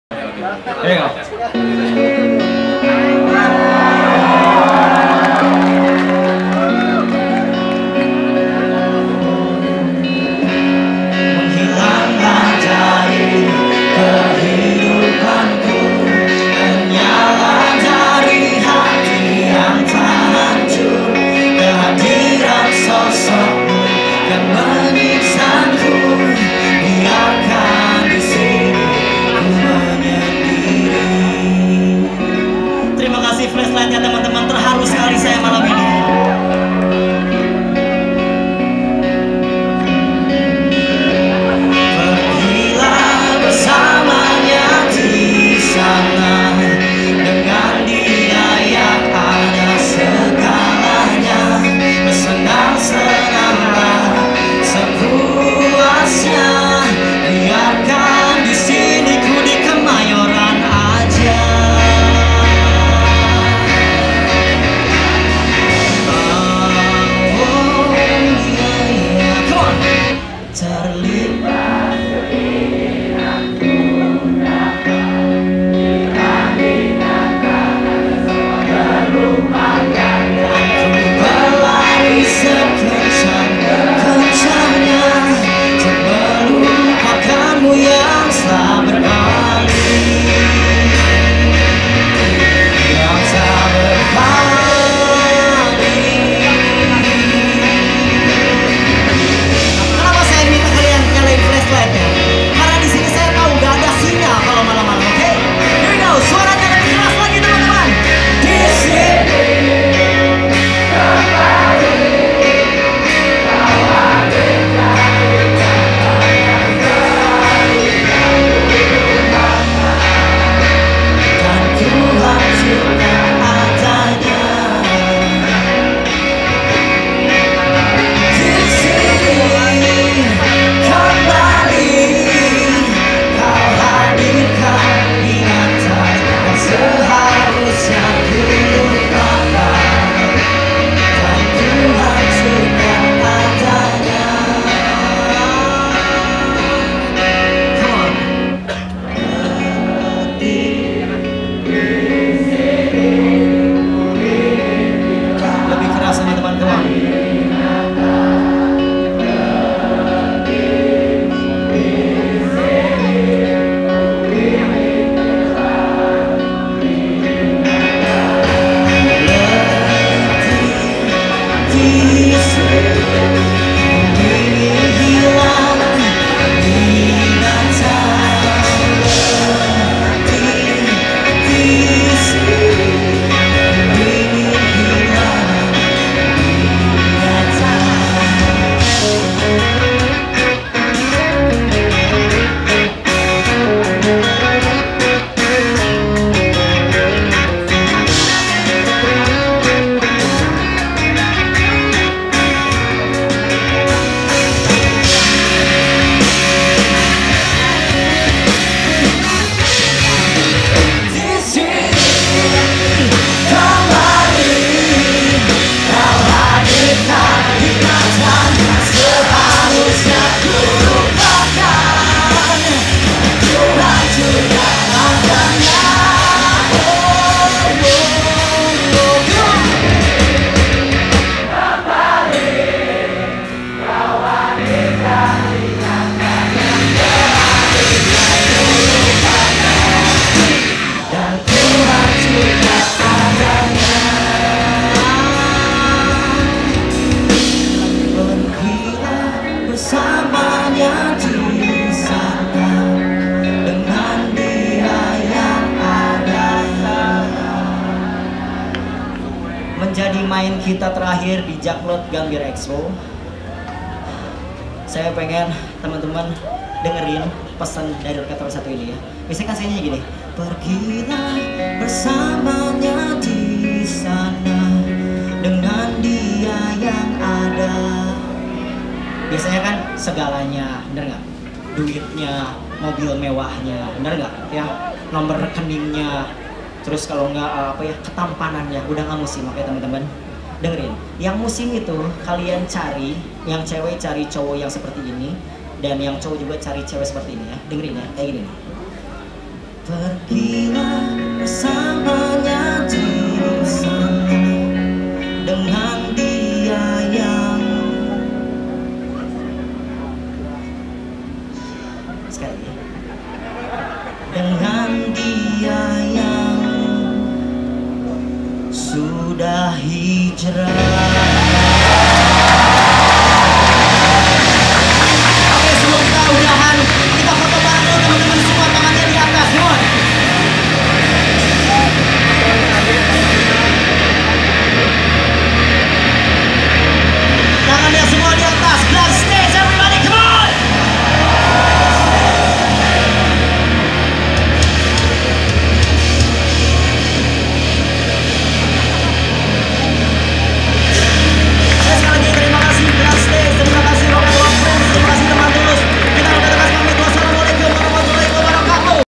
Rocket Rockers at Bloods Stage on Jakcloth Year End Sale 2017
Kemeriahan aksi Rocket Rockers di panggung Bloods pada Jakcloth Year End Sale 2017 arena PRJ Kemayoran Jakarta